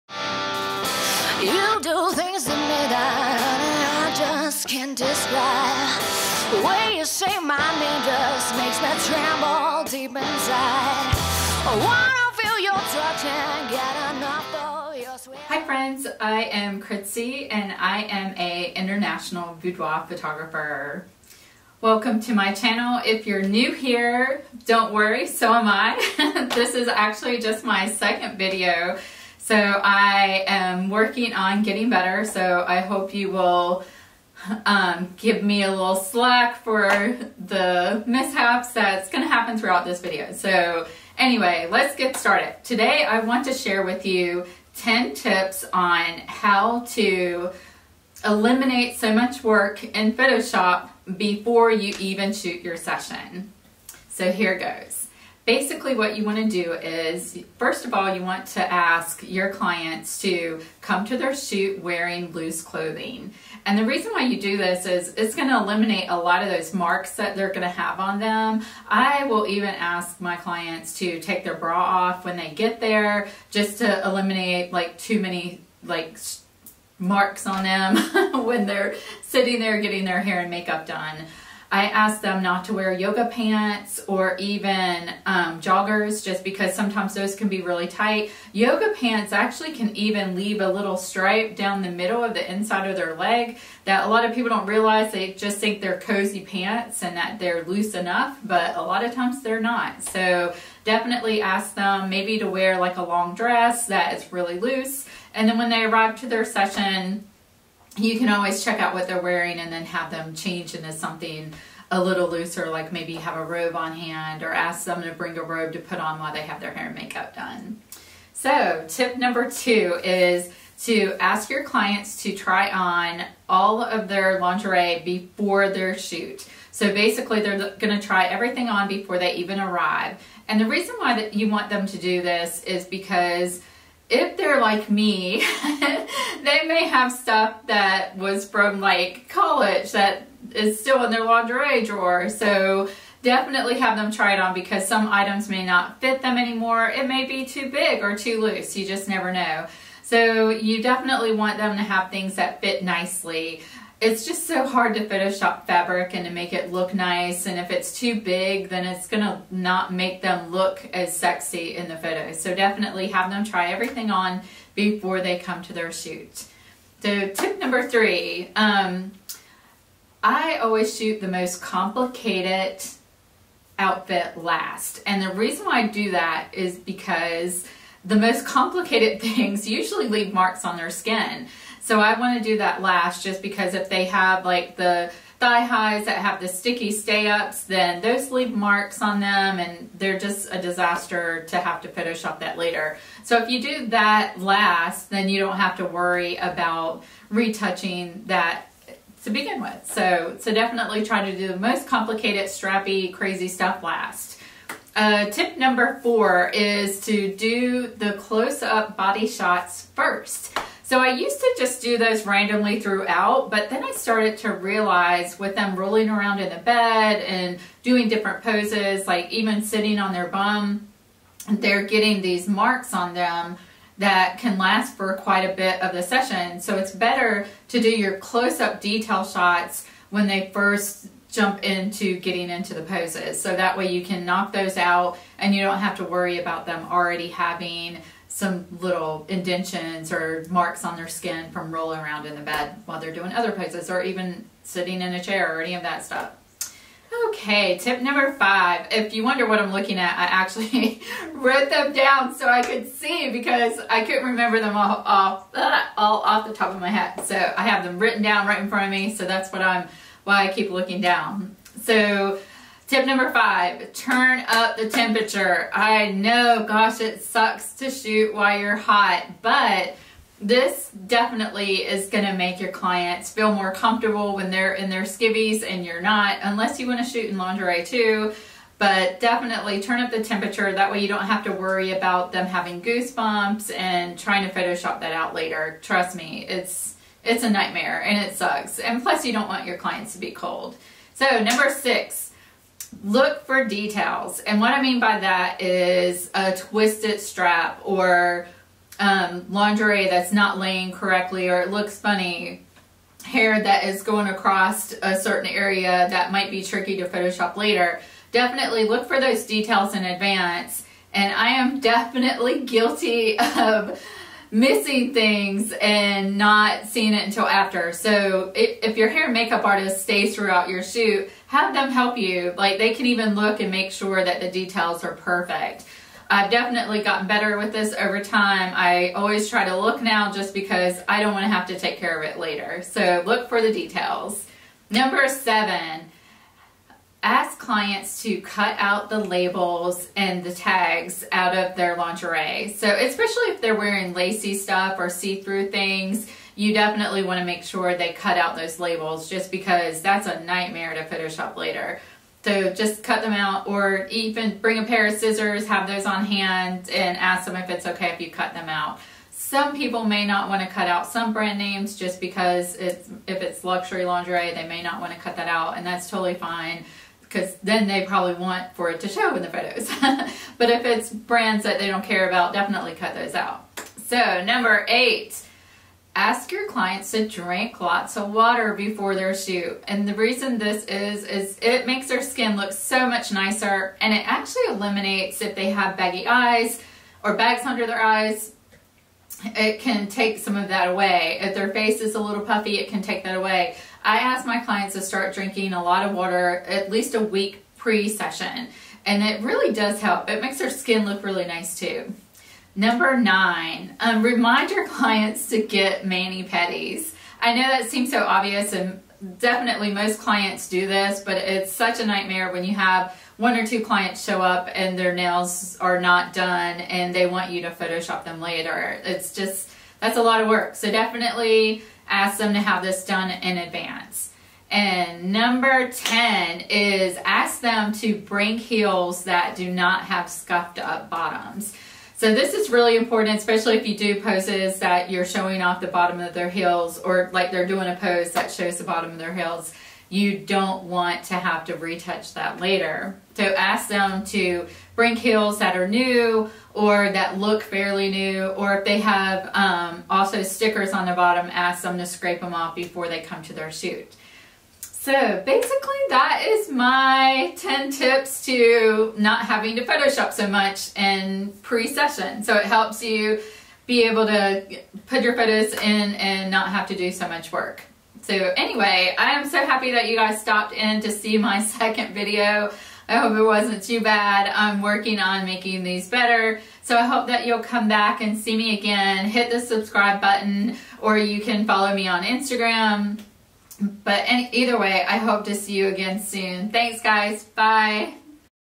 Ten Tips On Saving Time In Photoshop | Before Shooting Boudoir
Shooting a boudoir portrait session seems pretty simple... The client arrives, brings pretty things to wear, you shoot, all seems good. And then it gets complicated when it comes time to retouch, for one big reason. The subject is showing a lot more skin. There are very few people in this world that have perfect skin, so retouching boudoir is inevitable. However, there are tips and tricks I am sharing here to help eliminate extra retouching that you can avoid.
Watch this video to learn how to prep your clients to shrink editing time. When you prepare your clients by giving a few simple suggestions and taking some steps during the session, you can eliminate extra Photoshop time.

What's in my bag when shooting boudoir:
Sony A7R III Camera
Sony 24-70mm 2.8 GM Lens
Sony 50mm 1.8 Portrait lens
Sony 85mm 1.4 Lens
I will be releasing a video soon on lighting and will include my lighting systems then.